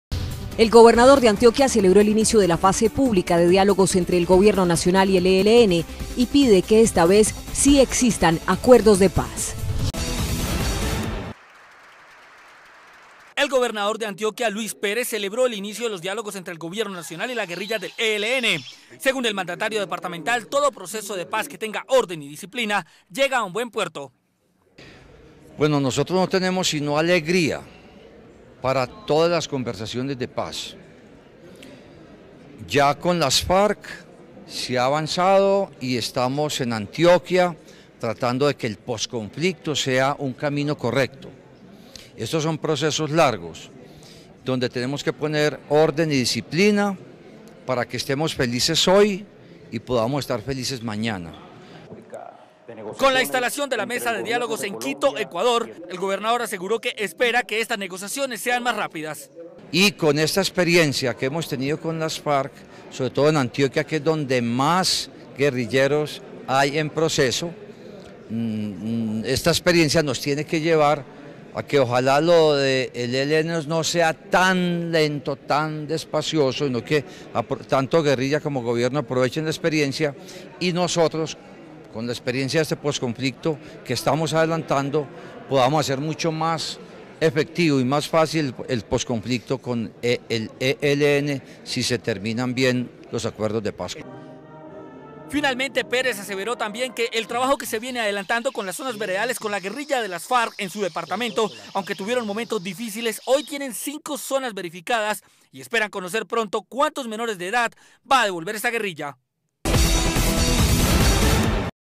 Gobernador de Antioquia celebra inicio de diálogos entre Gobierno y ELN
VIA: TELEANTIOQUIA